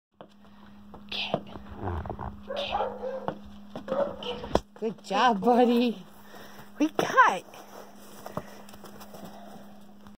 Video of adoptable pet named Trooper
Trooper is an adoptable pet with The Last Resort in Rockaway NJ.